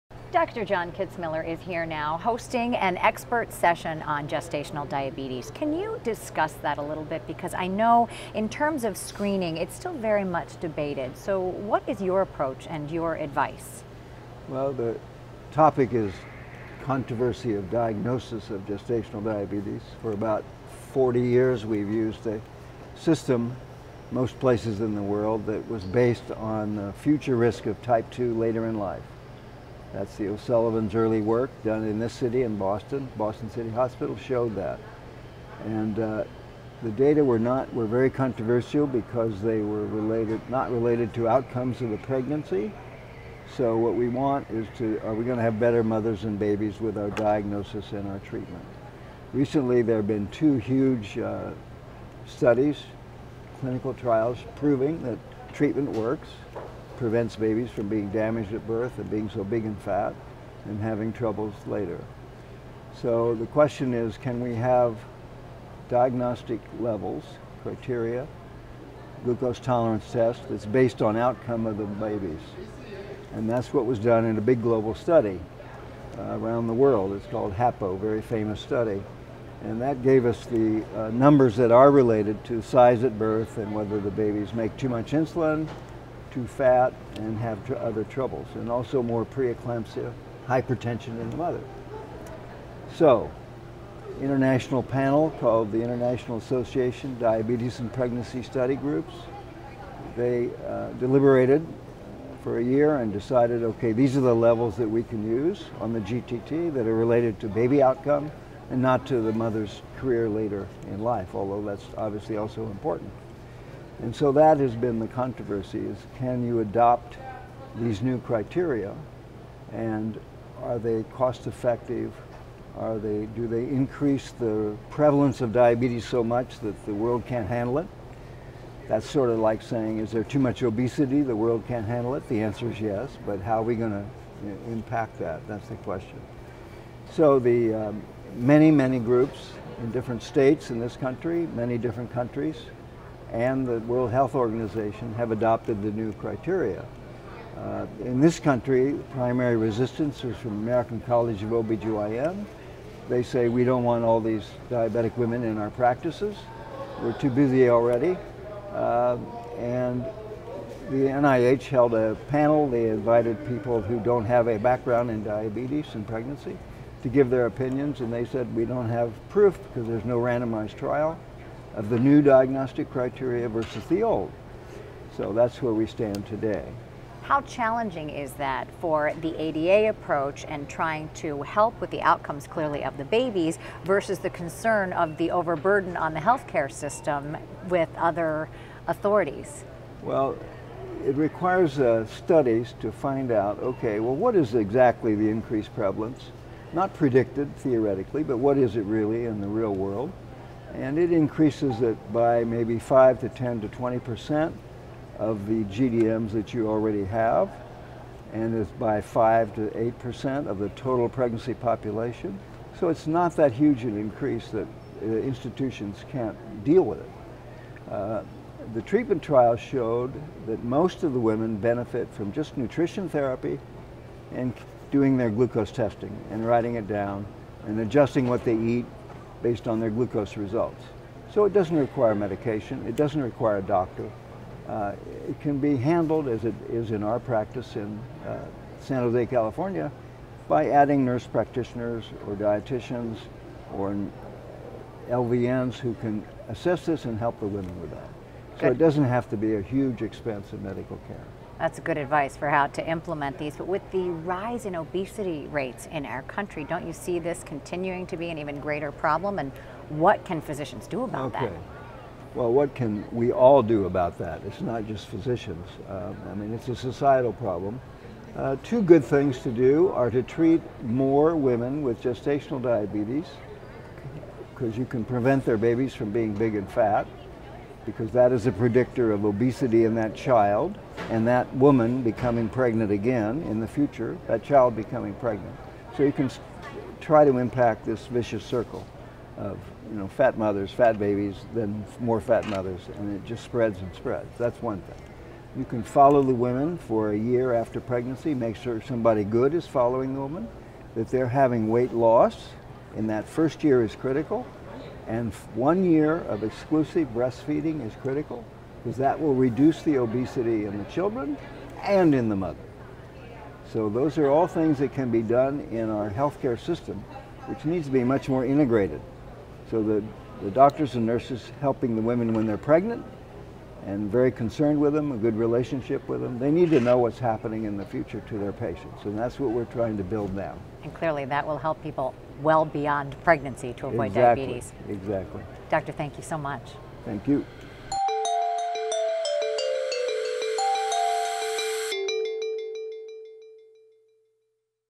Diagnosing Gestational Diabetes Mellitus - Still Controversial
John L. Kitzmiller, MD talks to ADA TV about this important Meet-the-Expert session, on Diagnosing Gestational Diabetes Mellitus, at the 75th ADA Scientific Sessions.